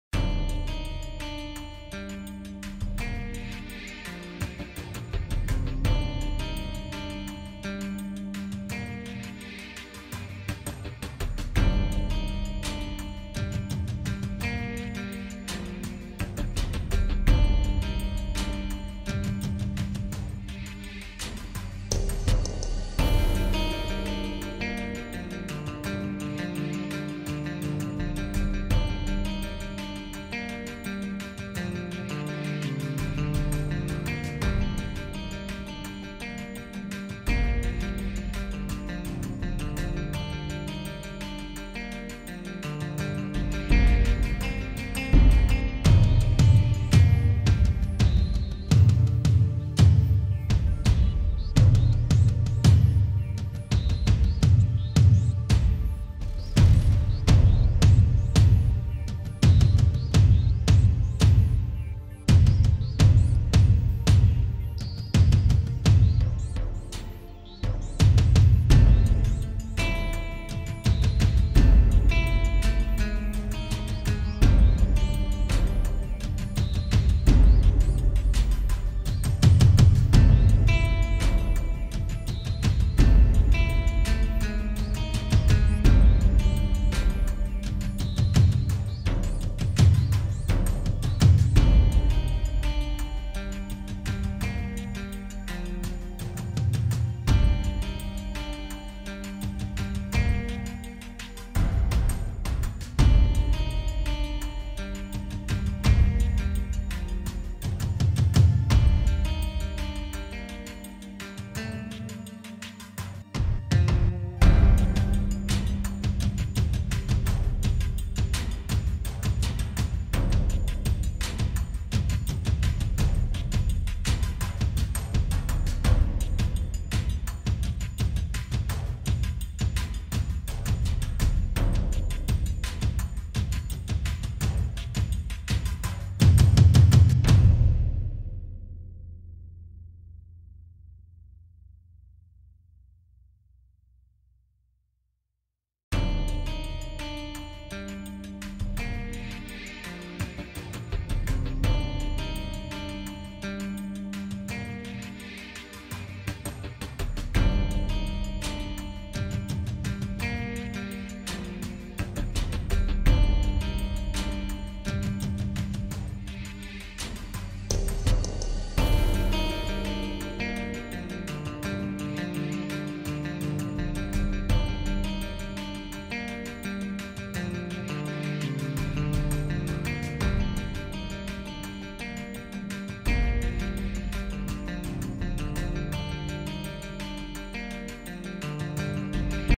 The Question Sketch Commission
This is a commission I made of the DC Comics character called The Question. I hope you like it! Remember to plz like and sub. Thanks!